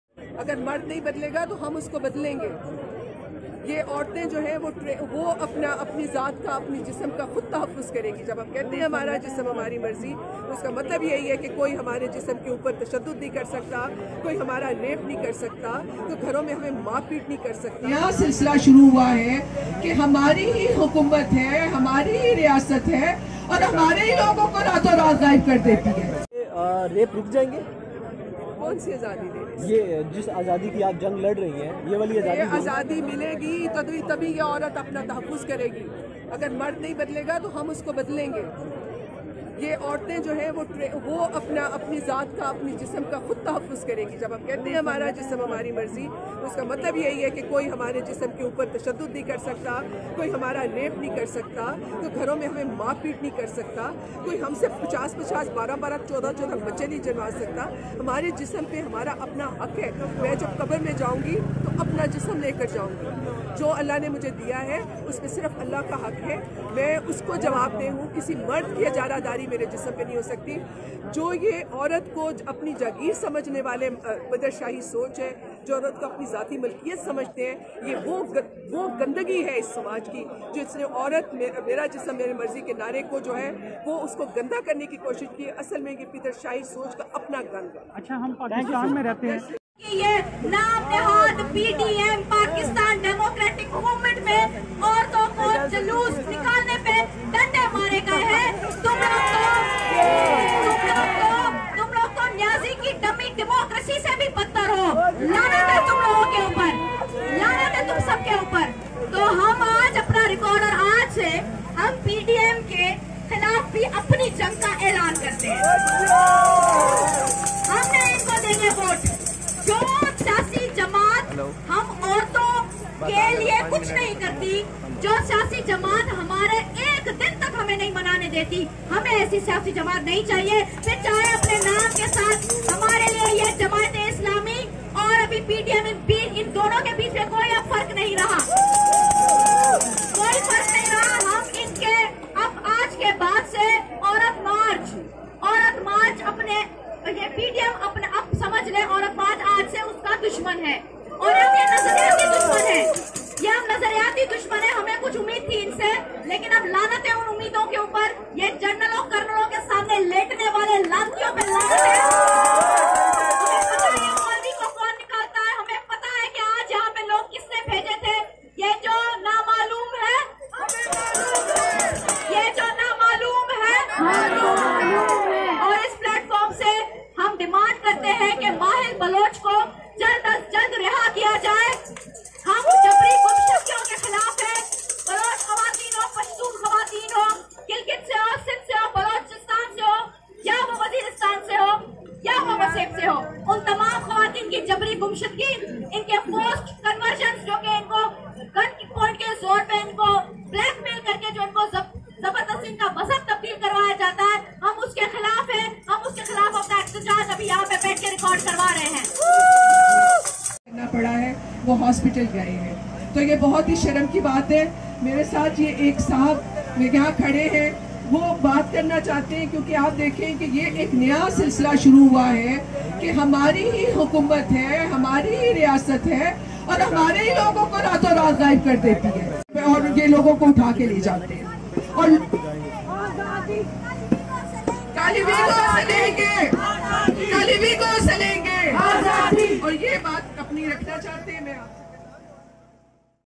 auratmarch2023 || Our state is lifting us
Aurat March was organized in Islamabad press club then started moving to D Chowk in a rally. they demands free space for women and stop rape, assault, and misbehave towards women